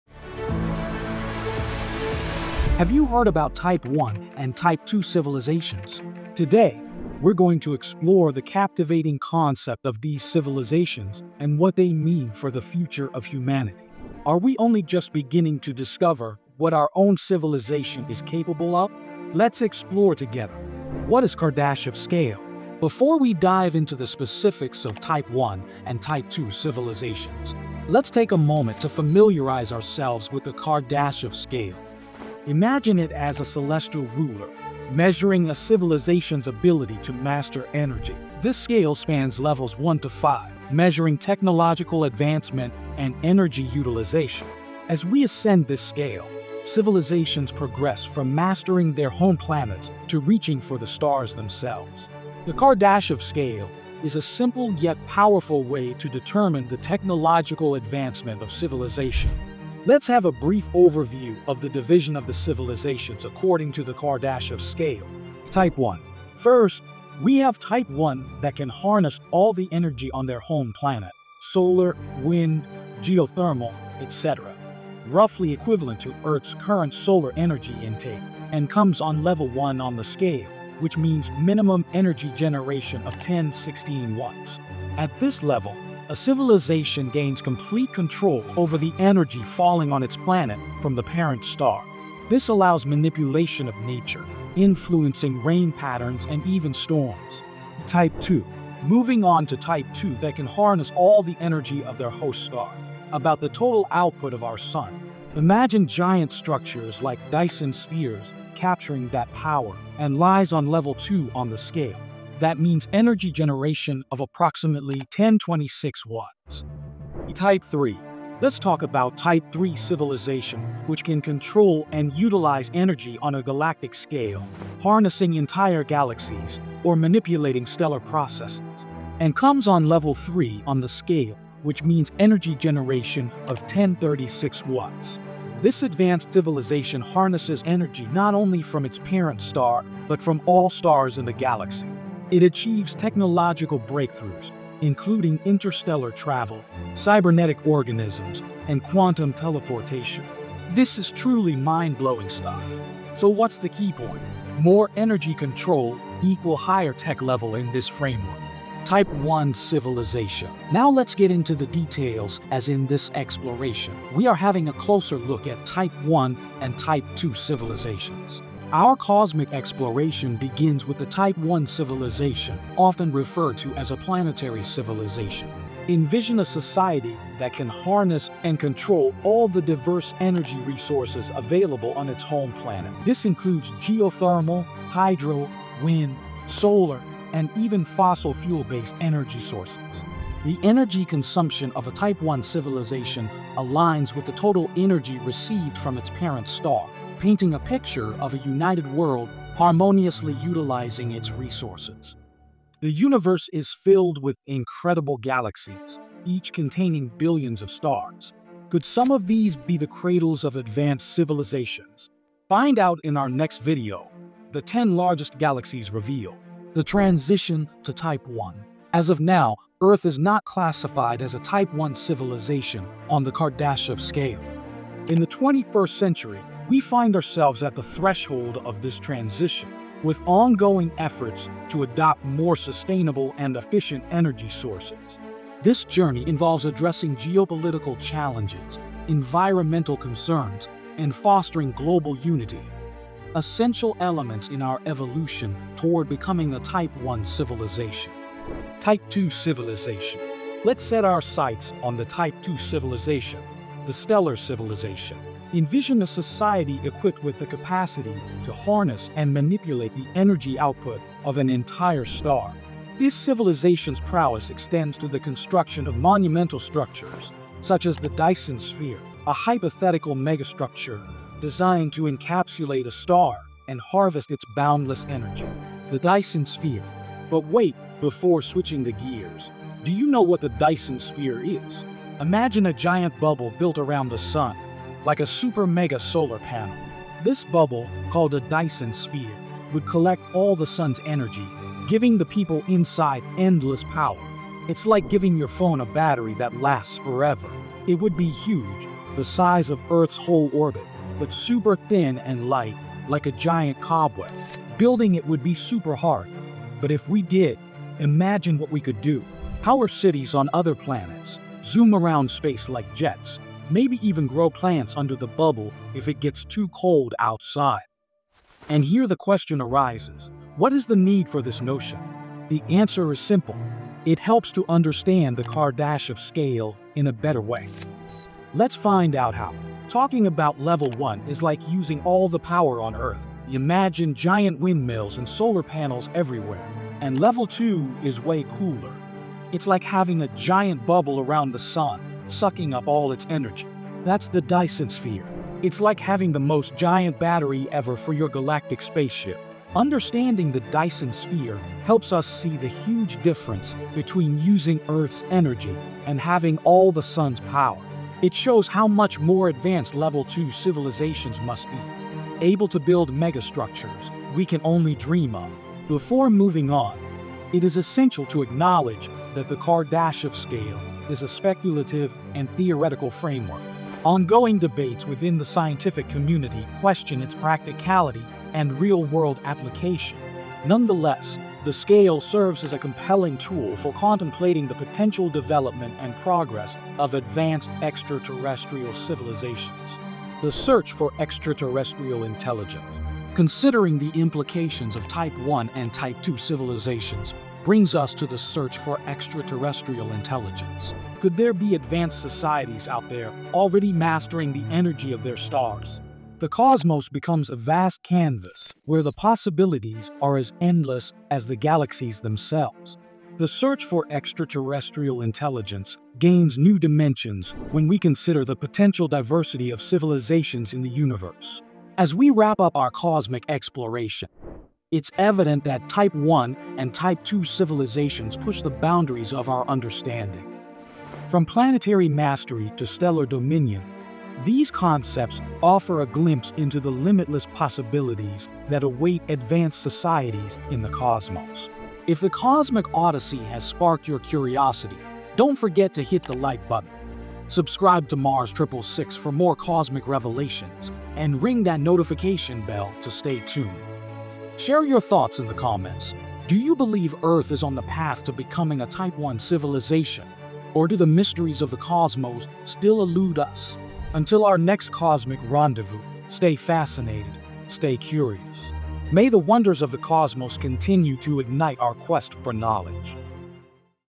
What Type I & II Civilizations Can DO? Are We on the Right Track?
Join us as we embark on an exhilarating journey into the realms of Type I and Type II civilizations, guided by the visionary Kardashev Scale! This cosmic classification system, developed by the renowned astrophysicist Nikolai Kardashev, outlines the stages of technological advancement and energy utilization that civilizations may achieve.

First, we explore Type I civilizations, those that have mastered the energy of their entire planet. Imagine harnessing the power of Earth's natural resources with extraordinary efficiency, such as controlling weather patterns, utilizing solar and geothermal energy, and even manipulating geological processes. This level of advancement represents a future where humanity can sustainably manage our world, creating an era of unprecedented technological and societal progress.

Next, we venture into the realm of Type II civilizations, those capable of harnessing the energy of an entire star! Picture constructing Dyson Spheres or Swarms around a star, capturing its immense energy output for various uses. Such advanced civilizations could create an entire system optimized for their energy needs, powering interstellar travel and immense technological feats that seem like the stuff of science fiction.

Sections Covered
0:00 Introduction
0:23 What is the Kardashev Scale?
1:01 Brief Overview of Civilization
2:40: TYPE I CIVILIZATION
3:24 Youtube Card
4:10 TYPE II Civilization
4:37 Dyson Sphere: A megastructure for stellar energy.
5:29 Level 1 vs. Level 2 civilizations.
6:07 Theoretical Nature of the Kardashev Scale
6:31 The Search for Extraterrestrial Intelligence
7:02 Wrapping Up
Outro